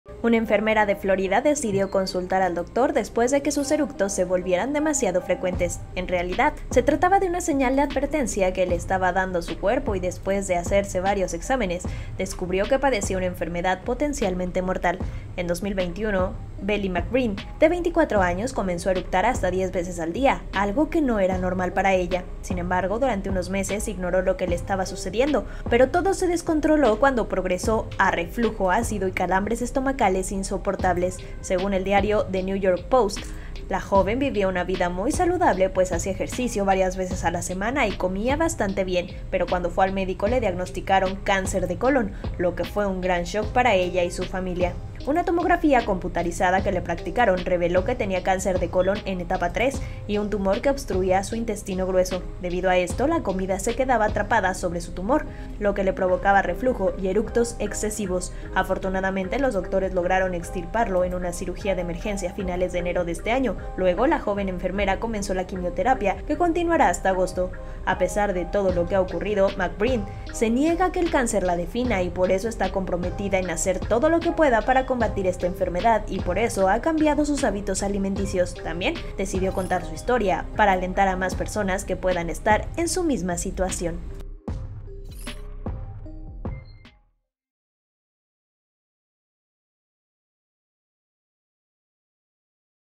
Por eructos frecuentes, mujer va a consulta y recibe diagnóstico devastador
Bailey McBreen padecía también reflujo ácido y un dolor ‘insoportable'